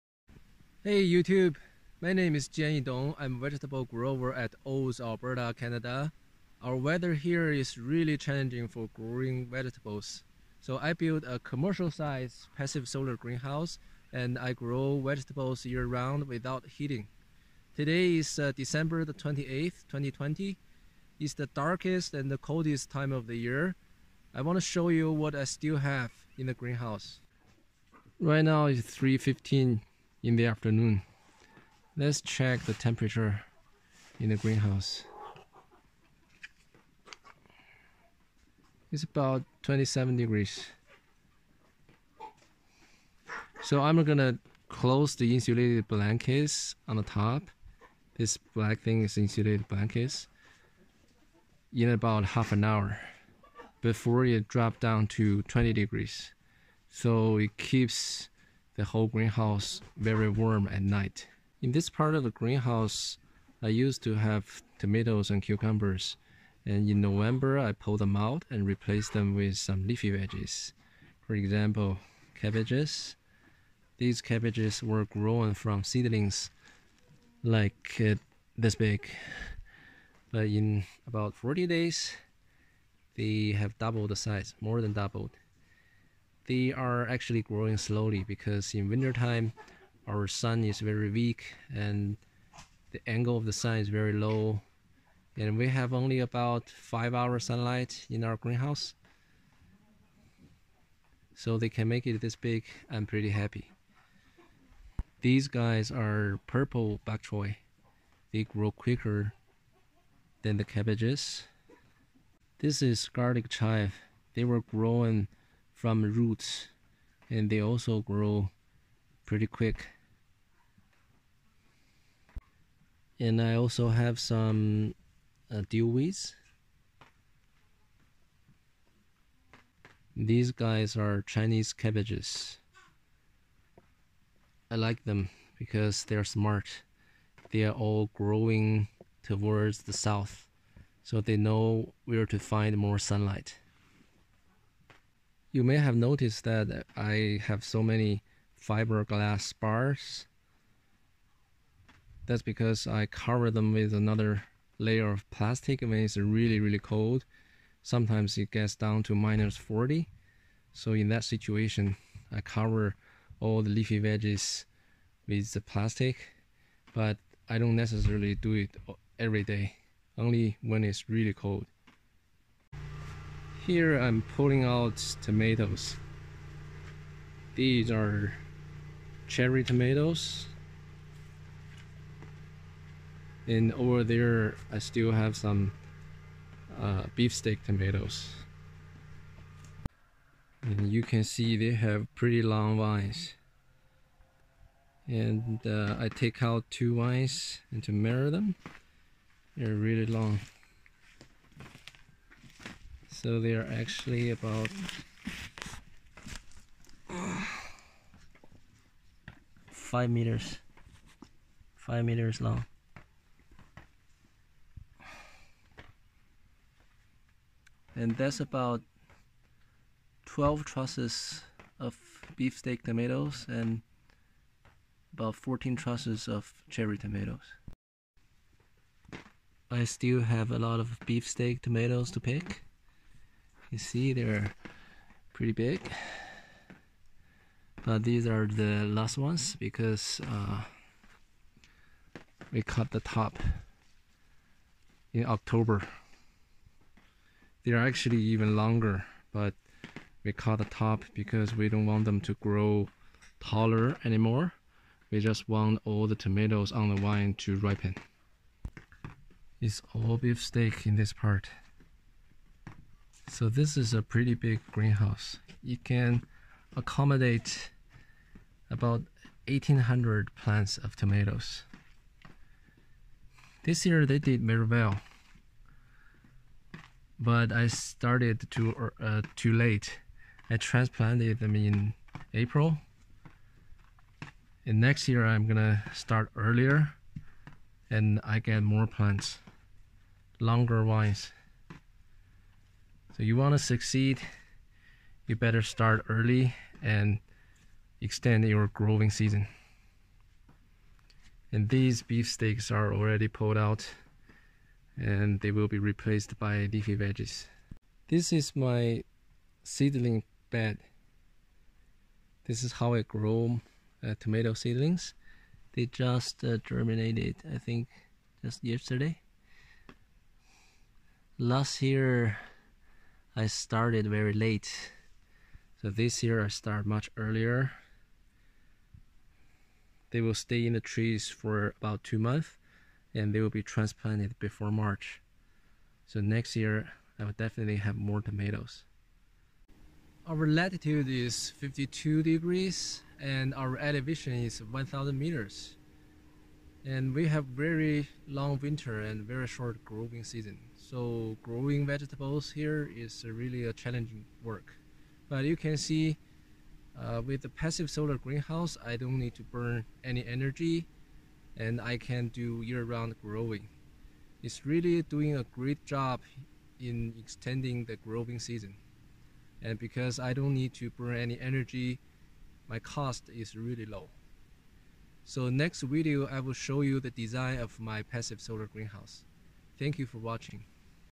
Growing year round in a passive solar greenhouse without heating
I am growing year round at Olds, Alberta, Canada. Our latitude is 52deg and elevation is 1000m. The passive solar greenhouse does a great job in extending the growing season in such a challenging place. No artifical heat source is used. The only heat source is sunlight. Passive solar greenhouses are very cost effective and environmentally friendly solutions to vegetable growing. Video taken on Dec 28th, 2020